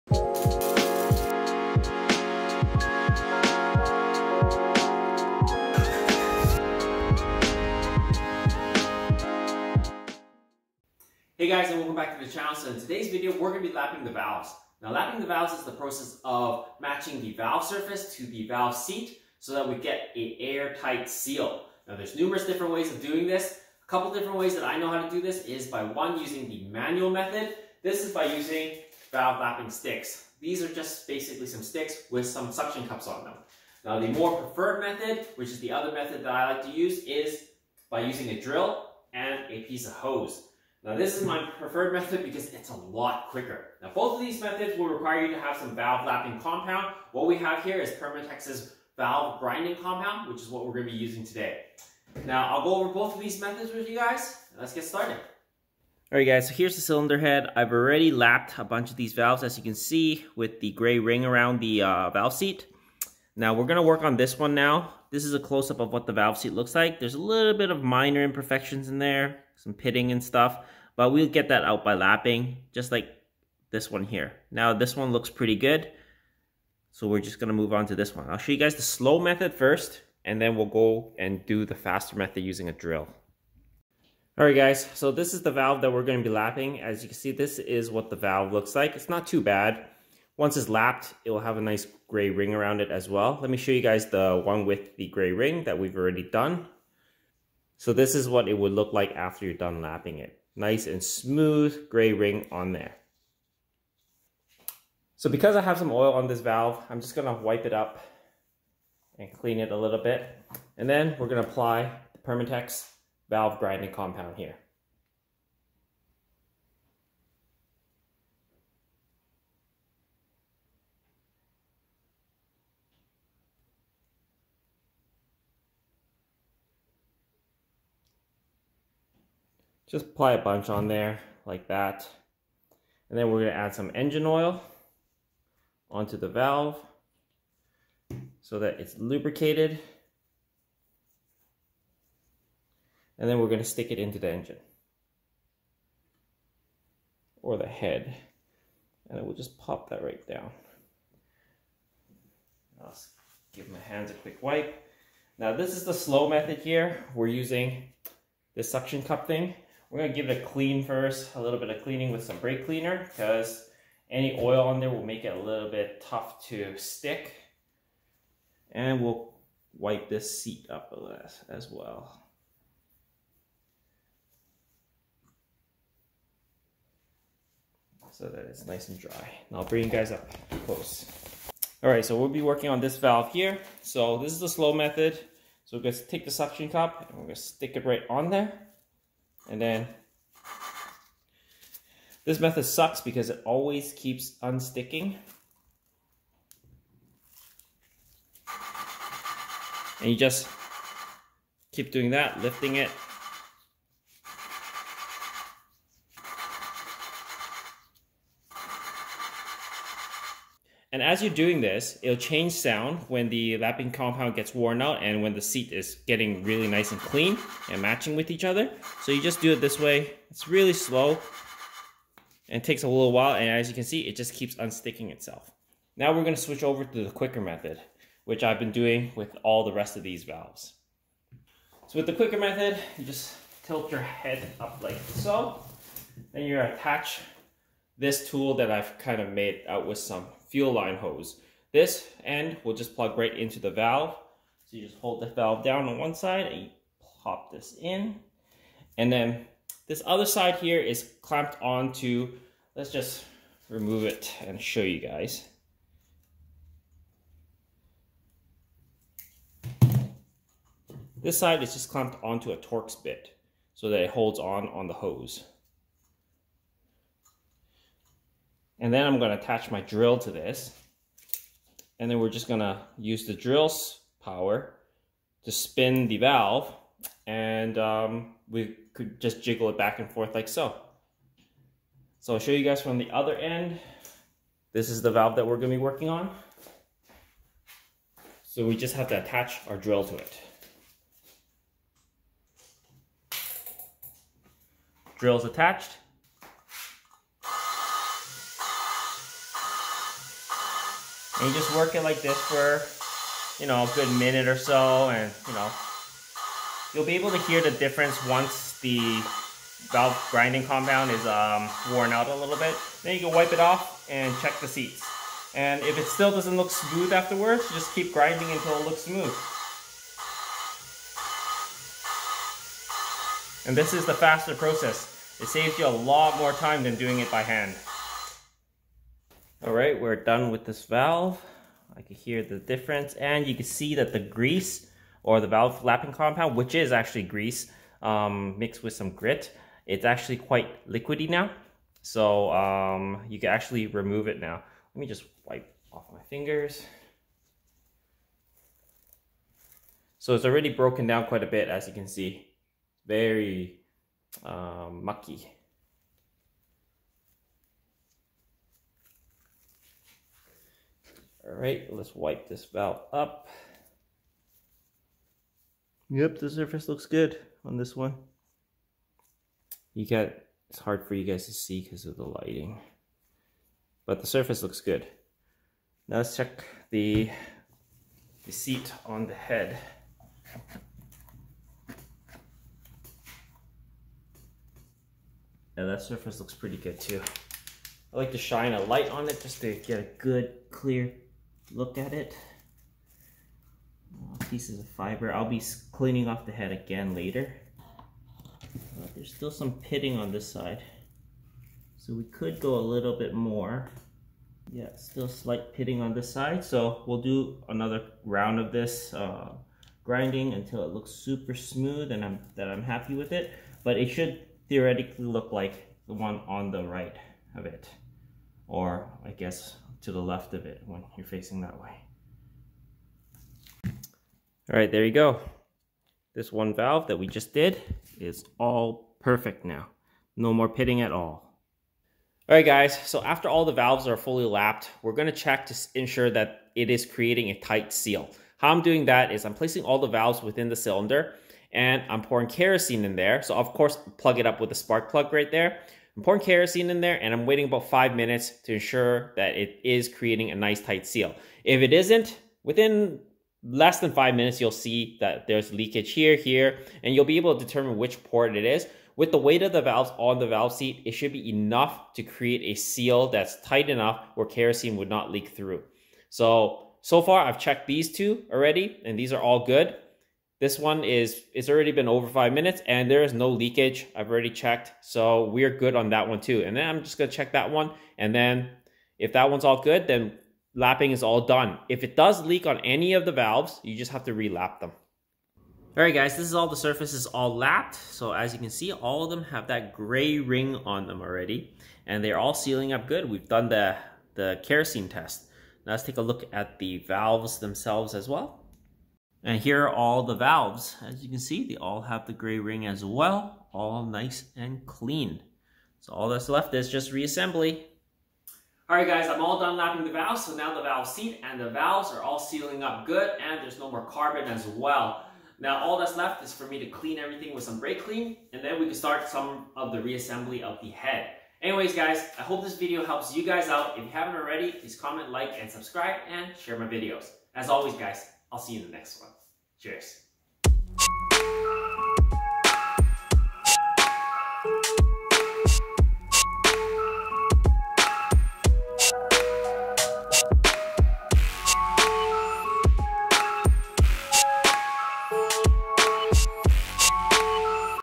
||HONDA EF9/ED7 CIVIC & CRX|| B18B1 Lapping Valves
This video covers:
▶ Different ways in lapping the valves
▶ How to lap the valves using the manual method
▶ How to lap the valves using a drill
▶ How to check valve and valve surfaces after lapping
▶ How to check the valves for an air tight seal

In this video, I go through steps on how to lap the valves in my B18B1. I also go through how to check for an airtight seal.

Any more questions about this video or suggestions for other DIY budget-friendly car mods/maintenance?